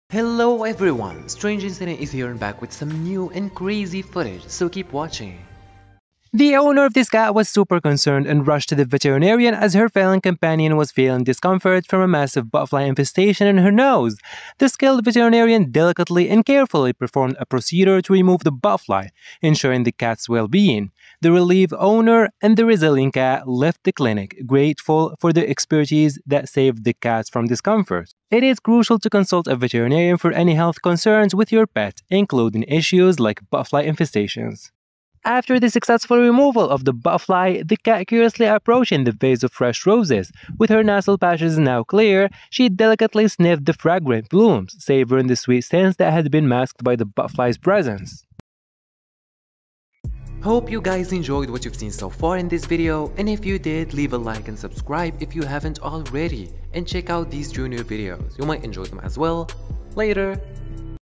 Botfly Maggot Removal From A Tiny Cat's Nose
See The owner of this cat who was super concerned and rushed to the veterinarian as her feline companion was feeling discomfort from a massive Botfly infestation in her nose. The skilled veterinarian delicately and carefully performed a procedure to remove the Botfly, ensuring the cat's well-being. The relieved owner and the resilient cat left the clinic, grateful for the expertise that saved the cat from discomfort.
it is crucial to consult a veterinarian for any health concerns with your pet, including issues like botfly infestations. Veterinarians have the expertise to diagnose and provide appropriate treatments, ensuring the well-being of your pet.
After the successful removal of the botfly, the cat curiously approaching the vase of fresh roses. With her nasal passages now clear, she delicately sniffed the fragrant blooms, savoring the sweet scent that had been masked by the botfly's presence.

See more lovely and beautiful videos from Katelyn Young, the owner of the cat with Botfly Maggot here: